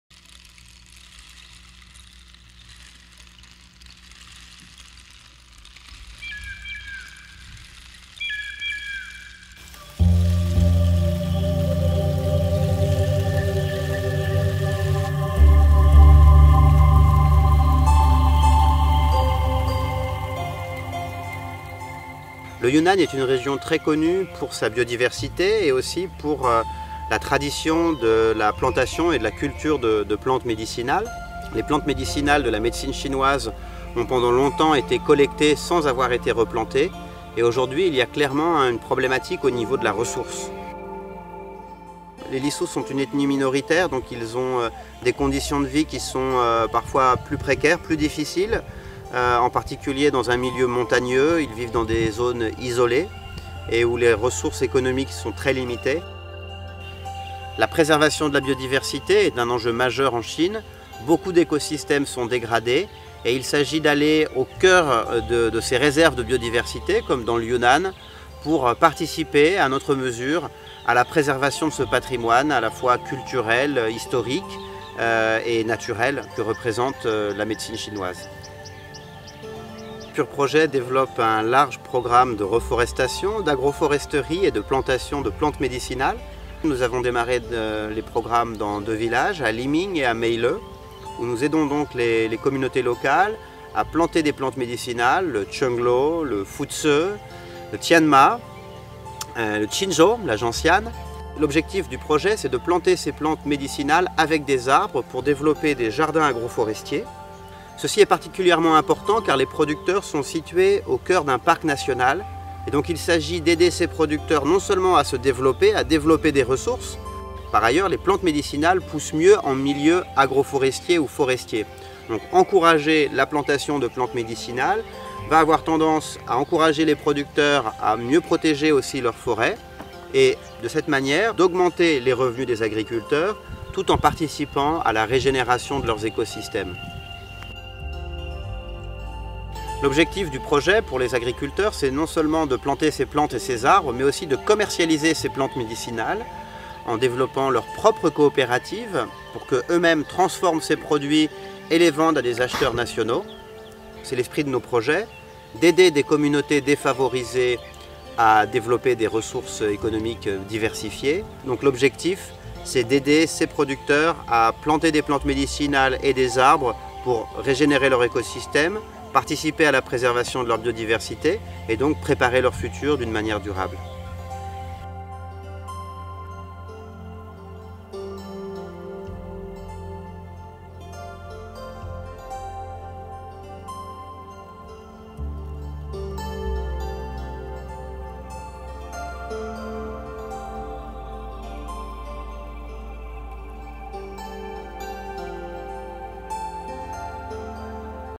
LIMING PROJECT CHINA - Fondation Crédit Agricole Suisse
Pur Projet develops a large-scale reforestation, agroforestry and herbal planting in the Yunnan region of China. This area is well-known for its great biodiversity as well as its herbal plant cultures, traditionally used for Chinese medicine. The goal is to protect and regenerate the local ecosystems while helping farmers progress, preserve and perpetuate their long-term tradition.

The project is supported by Fondation Crédit Agricole Suisse.